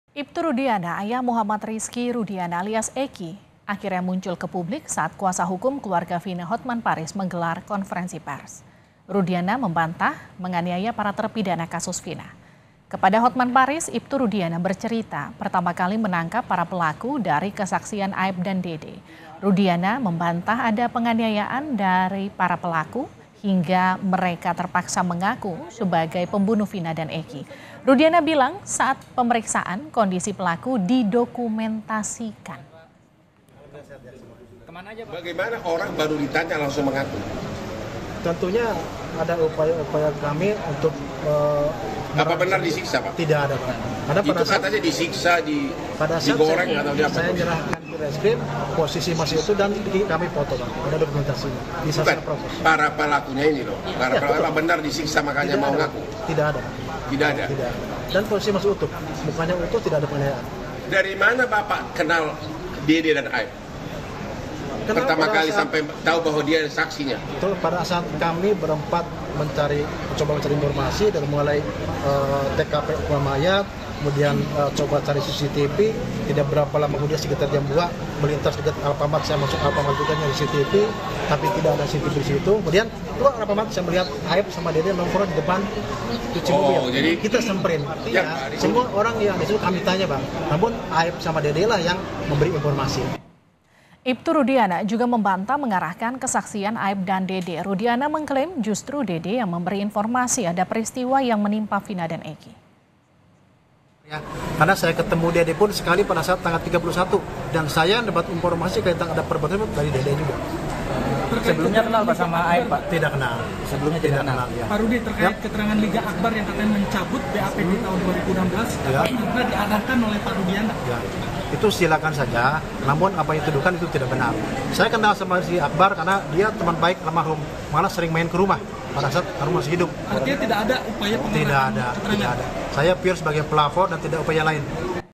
Bicara Soal Kasus Vina Cirebon, Iptu Rudiana Bantah Aniaya Para Terpidana hingga Arahkan Saksi
CIREBON, KOMPAS.TV - Iptu Rudiana, Ayah Muhammad Rizky Rudiana alias Eky akhirnya muncul ke publik saat Kuasa Hukum Keluarga Vina, Hotman Paris menggelar konferensi pers.

Rudiana membantah menganiaya para terpidana kasus Vina.

Kepada Hotman Paris, Iptu Rudiana bercerita pertama kali menangkap para pelaku dari kesaksian Aep dan Dede.

Rudiana juga membantah adanya penganiayaan pada para pelaku,hingga mereka terpaksa mengaku sebagai pembunuh Vina dan Eky.

Rudiana bilang, saat pemeriksaan, kondisi pelaku didokumentasikan.

Iptu Rudiana juga membantah mengarahkan kesaksian Aep dan Dede.

Rudiana mengeklaim, justru Dede yang memberi informasi adanya peristiwa yang menimpa Vina dan Eky.

Baca Juga Sidang Lanjutan PK Saka Tatal dengan Agenda Pembuktian, Apa Iptu Rudiana Akan Dihadirkan? di